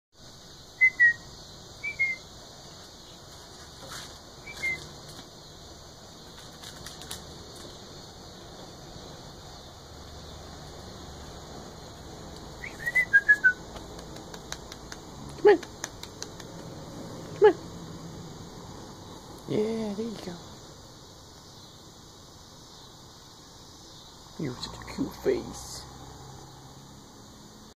Angelina - Female Goldendoodle w champion lines
This is Angelina at 9 weeks.  She is an extremely playful, good-natured goldendoodle pup.  Her father Romeo has AKC champions in his 2nd generation.